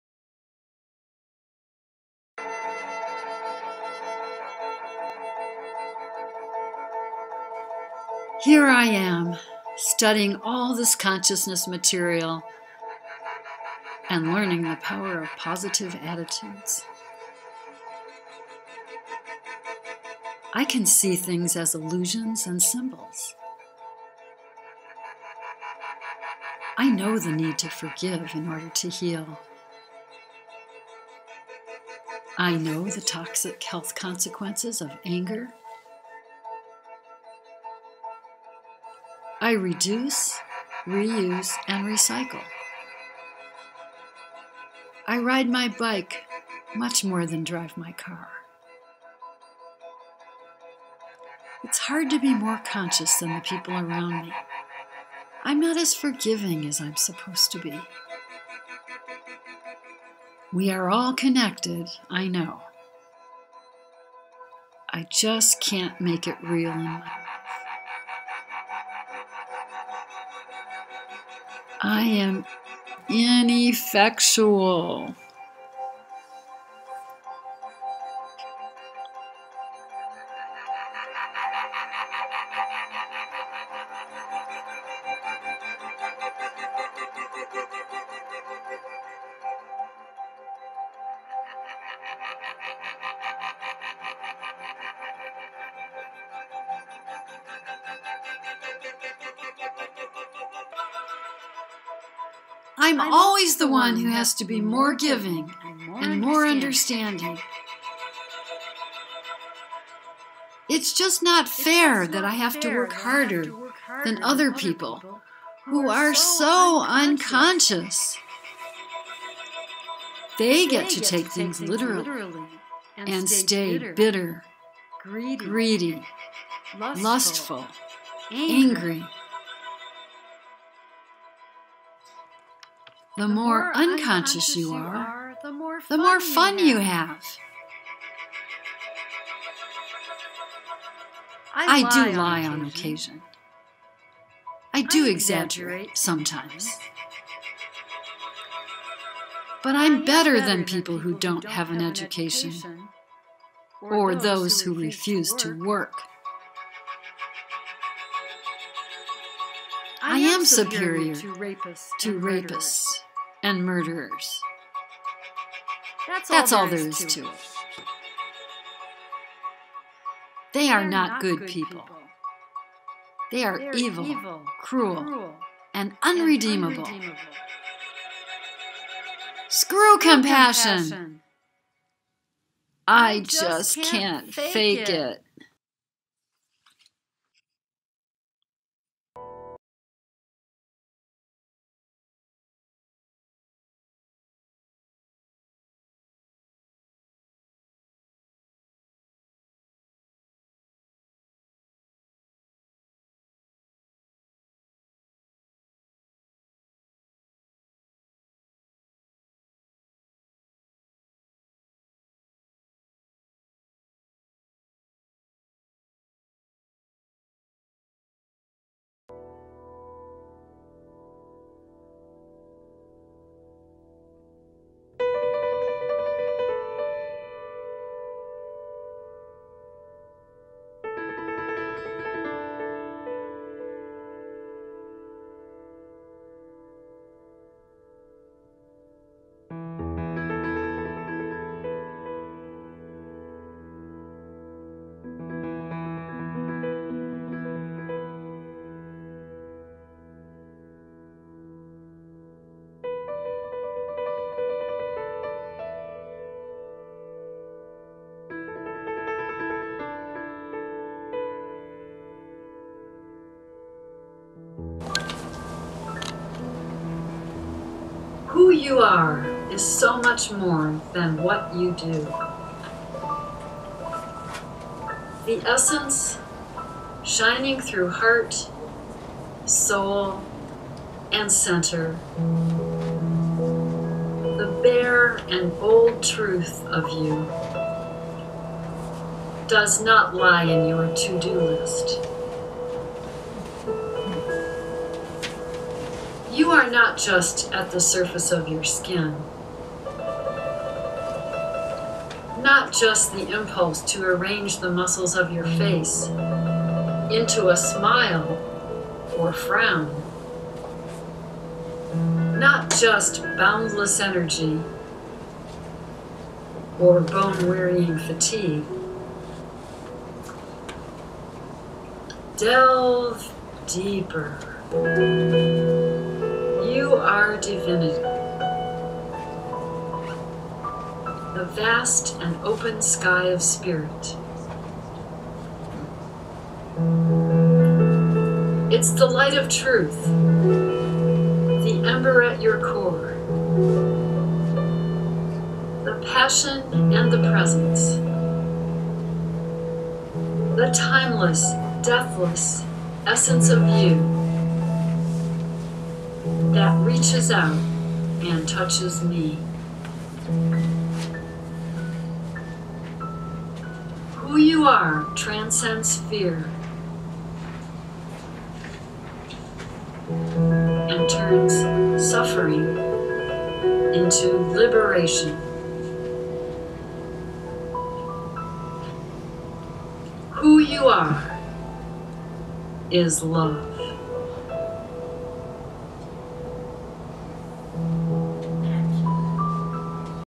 Who You Are
Concept and performance by Carol Reinhart, with text adapted from Carolyn Myss and poem by Danna Faulds.  Masks by Lauren Raine.
From the "TRUTH" show at the Carport Theater, May 19 and 20, 2018, in Tucson, AZ.  Video footage by Kathy Keler & Rosie Dempsey, editing by Kathy Keler